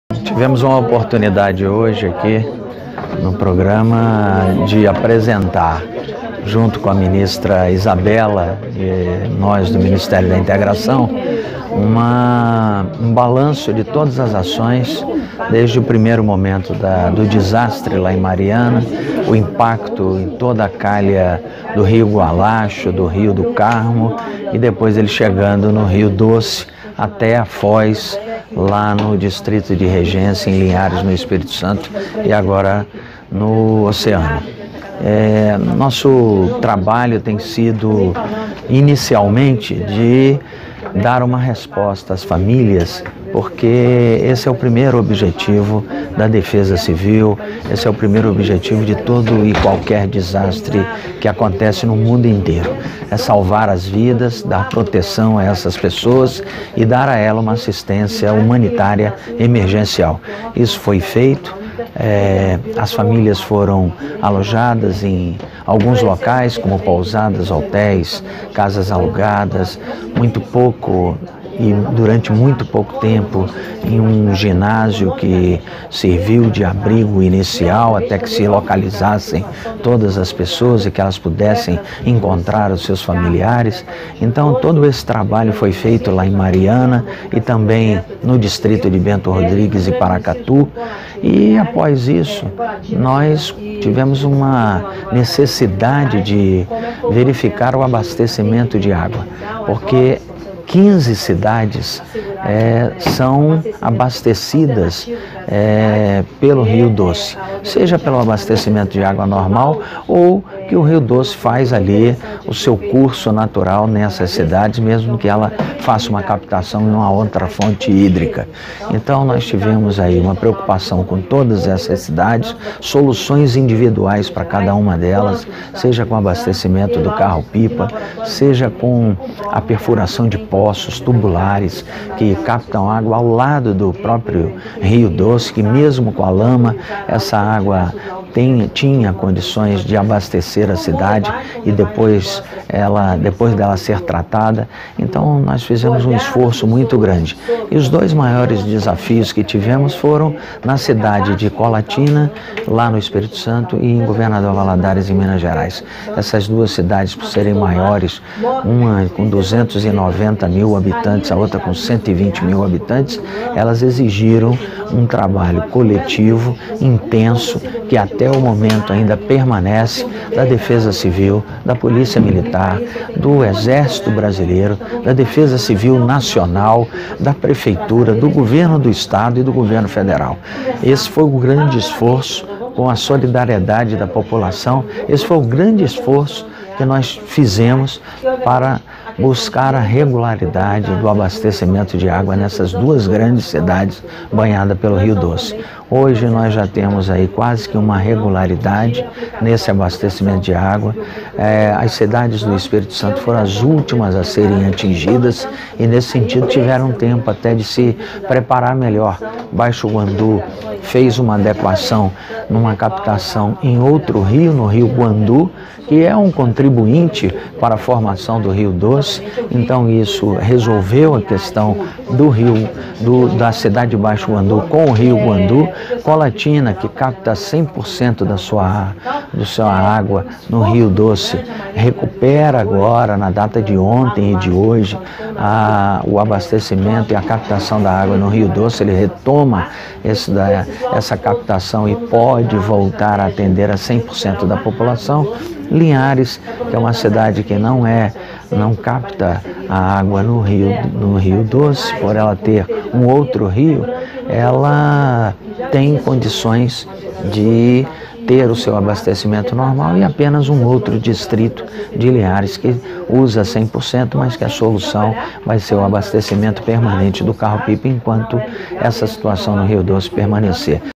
Gilberto Occhi explica ações para garantir abastecimento de água em cidades do ES e MG
ESPECIAL WEB - 25.11.15: Após participação no programa Bom Dia, Ministro desta quarta-feira (25), o titular do Ministério da Integração Nacional​, Gilberto Occhi falou com a TV NBR sobre o abastecimento de água nas regiões afetadas pelo rompimento de barragem em Mariana (MG) e as ações realizadas para garantir a segurança das vítimas.